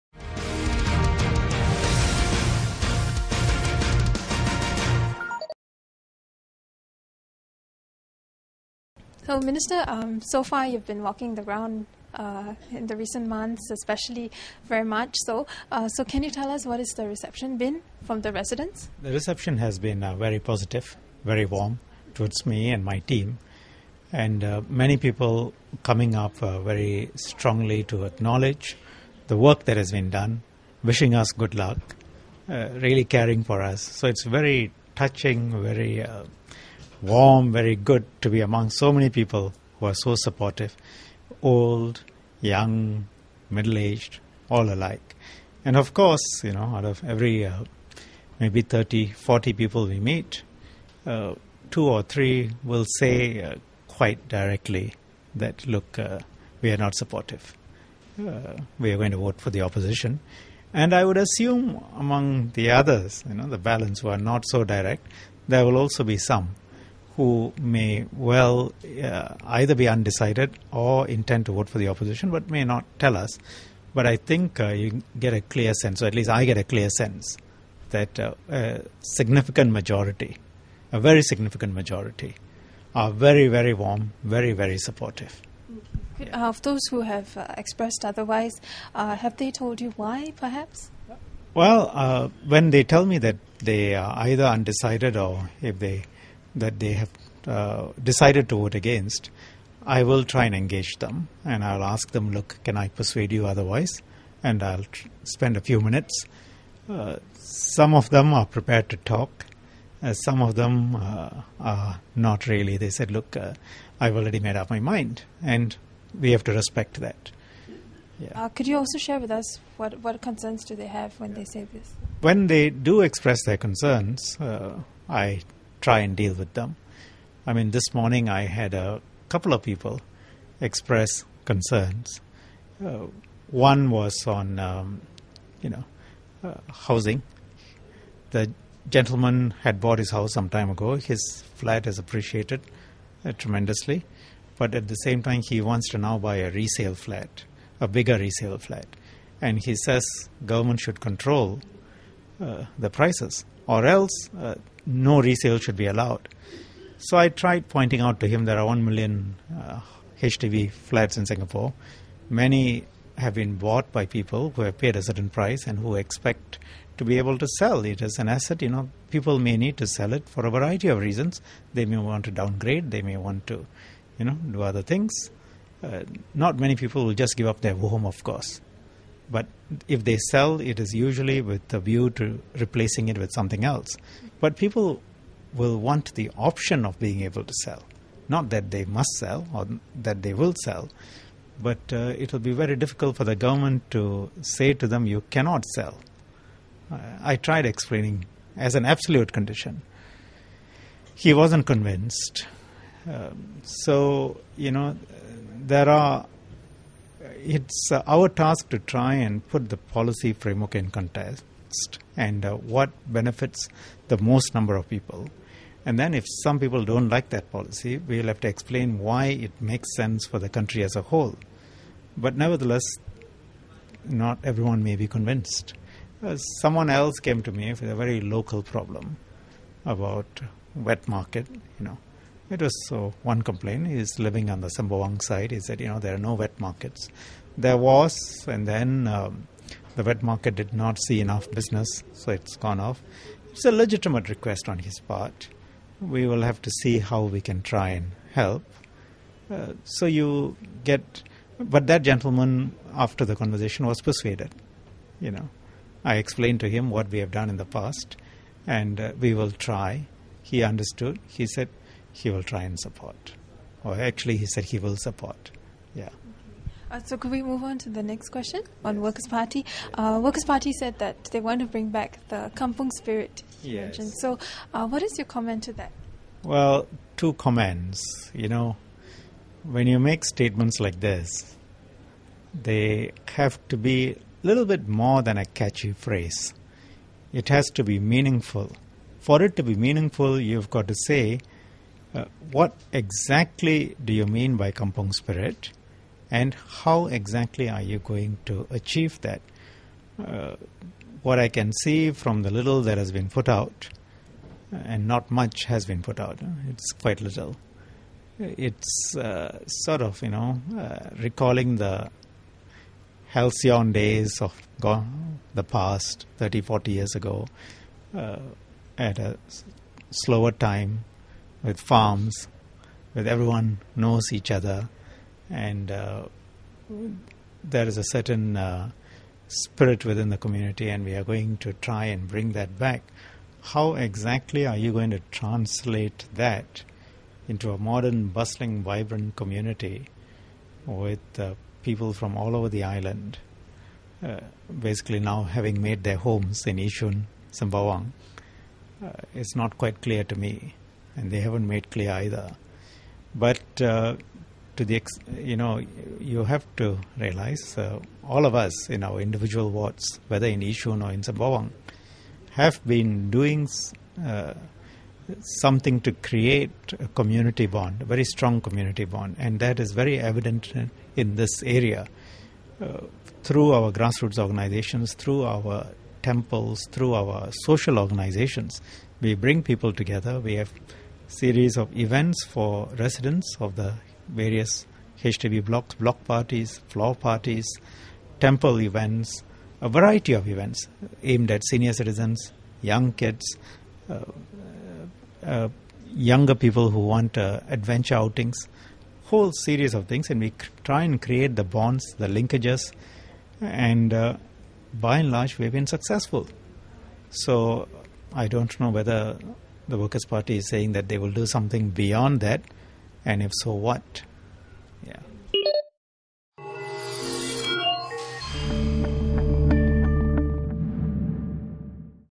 Interview with Minister Shanmugam @ Yishun
Minister for Law and Home Affairs K. Shanmugam gave an assessment of the reception he and his team have received during the walkabouts and public outreach leading up to the General Election.